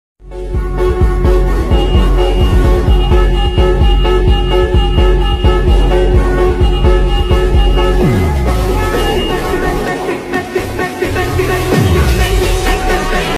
Jonathan bustos resmi ke Persija Jakarta musim depan
Jonathan bustos resmi ke Persija Jakarta musim depan briliga1 liga1bri jonathanbustos persija persijajakarta pssi fypシ briliga1update briliga12022_2023 briliga1⚽️